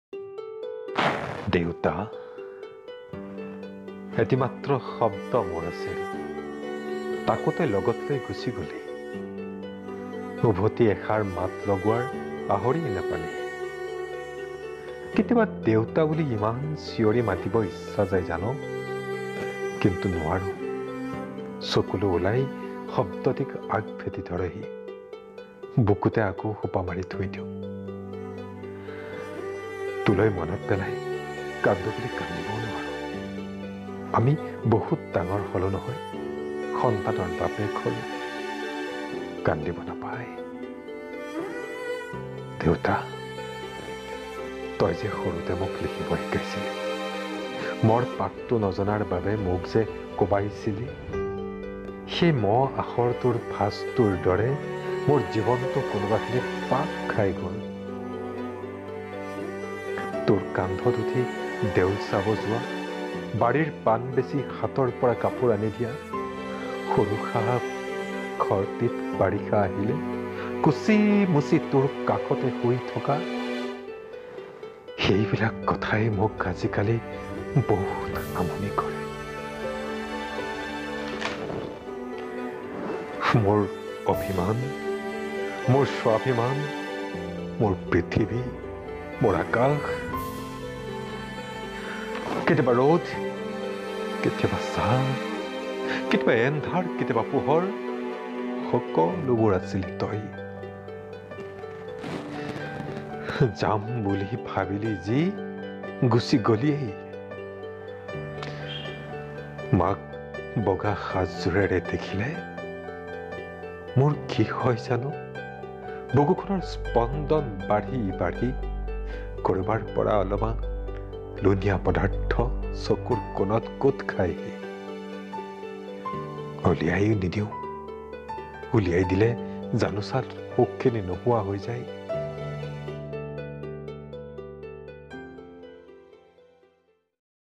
দেউতা (Deuta) ॥Assamese Sad Poem For Father
Hello friends

Please enjoy the assamese sad poem Deuta, Those who doesnot have father he can only realise the absence of father in life. Father has a great role in our life. I have missed my father so I can realise. Please love and care your Father. They need nothing from us except love and care from us. You are lucky because you have a father to guide you.